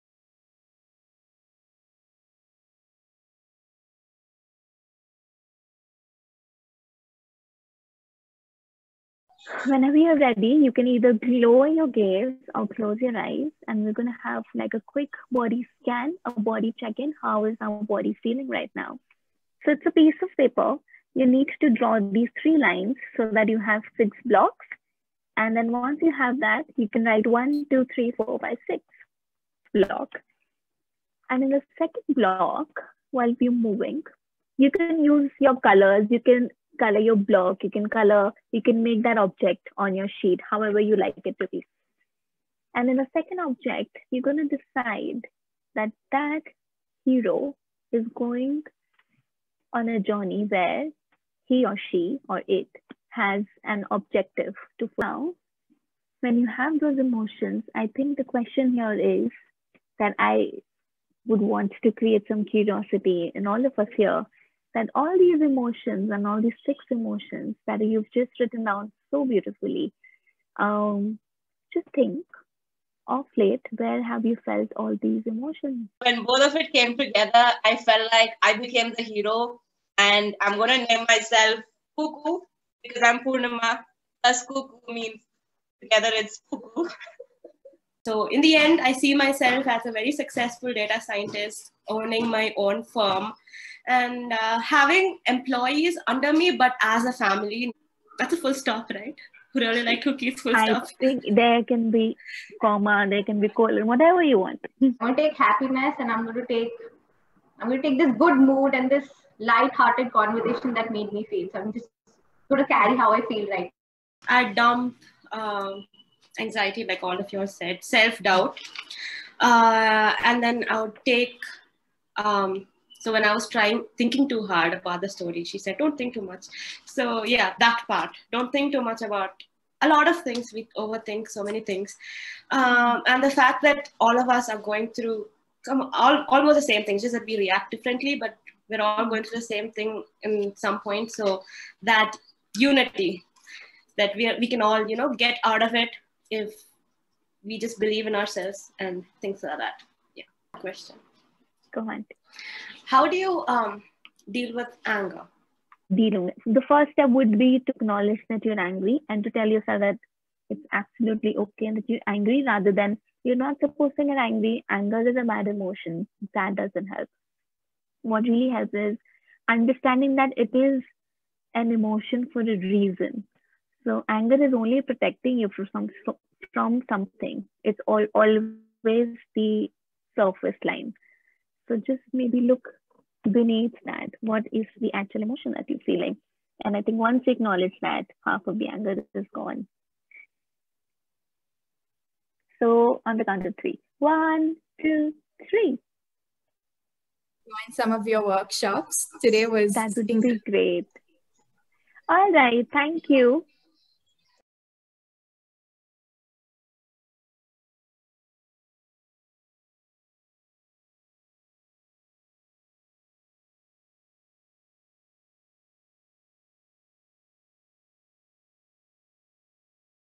Creative Arts Therapy with Rakshita Goel
A sneak peek into the fun taster dramatherapy session with founder of @tap.to.heal, Rakshita Goel @rakshitagoel22 on November 8th, 2020.